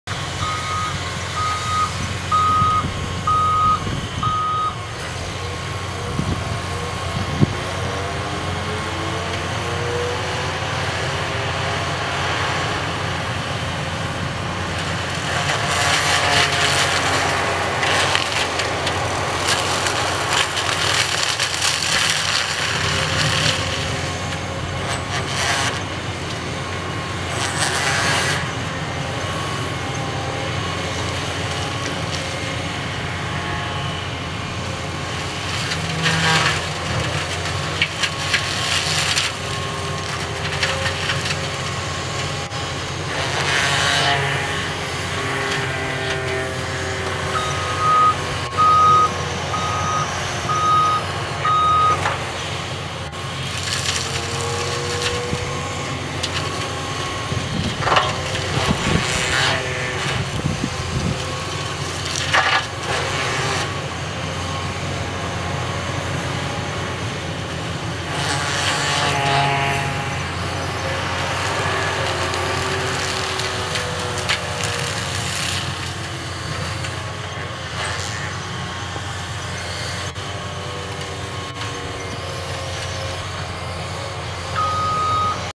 Cedar Tree Removal 2
Removing cedar tree with saw on excavator.